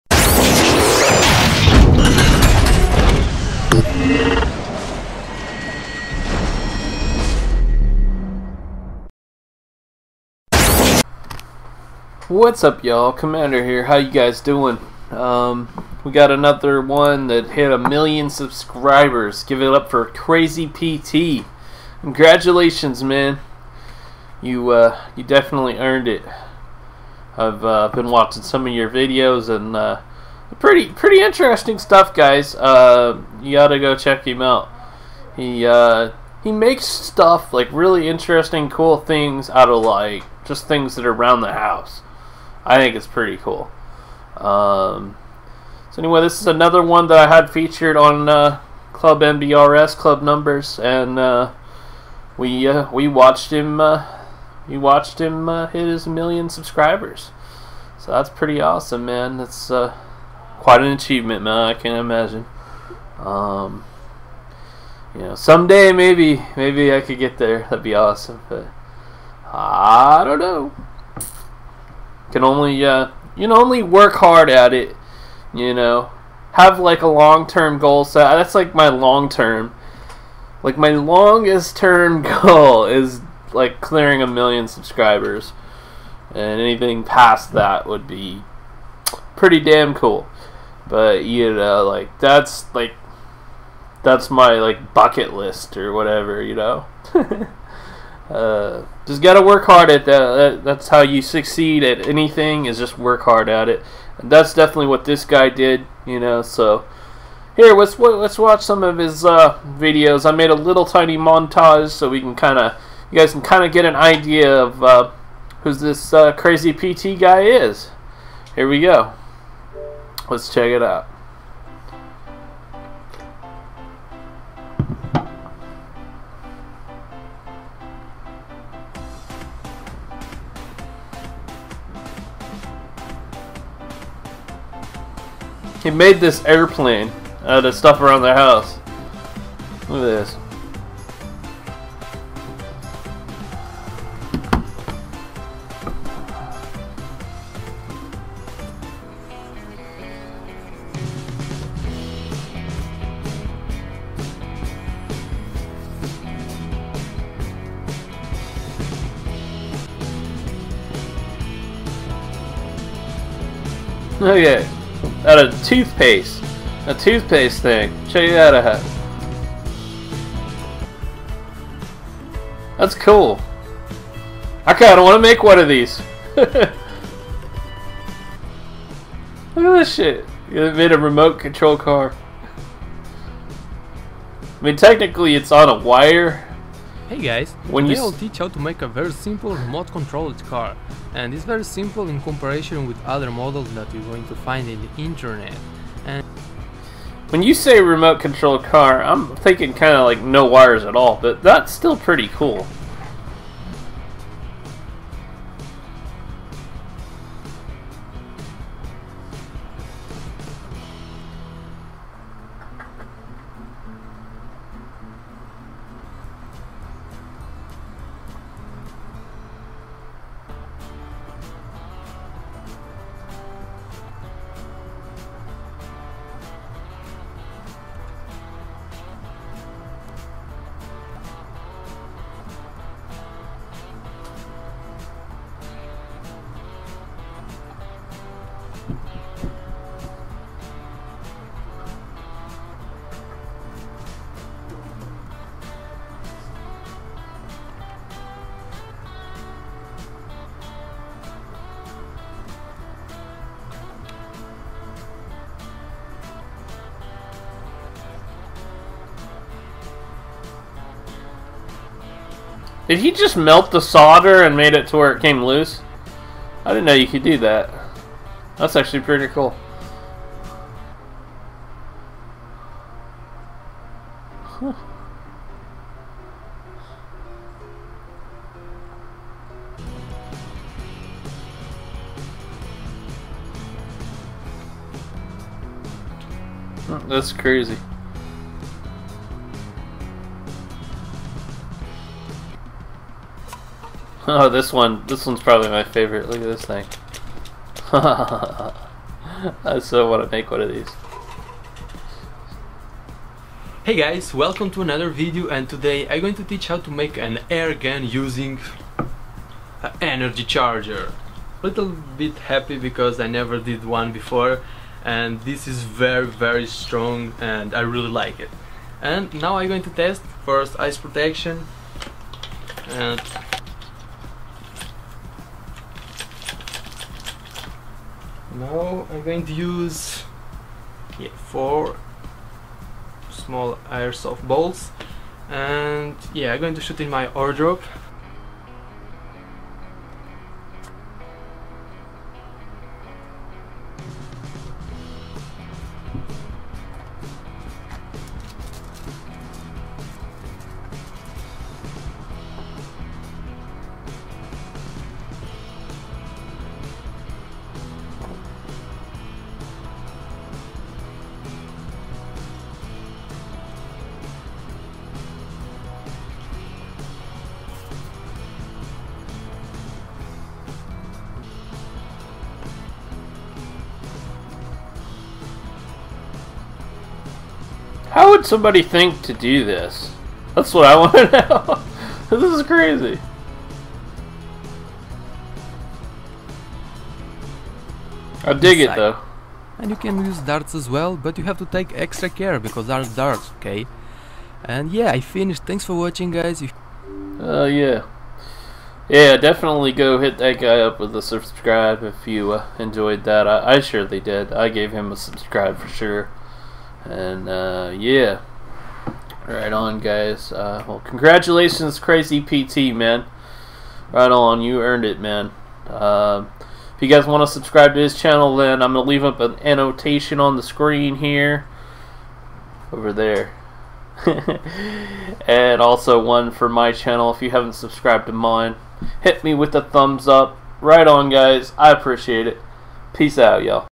Crazy PT - Hits 1 Million Subscribers
This is the third episode of our new series called the 1 Million Subscribers Club.
Crazy PT Makes really cool thing from materials & things found around the house. He makes things with household items. I have found this channel very interesting and fun to watch. Some of the things he made, I want to try and make myself.

NBRS_COMMANDER
 le commandant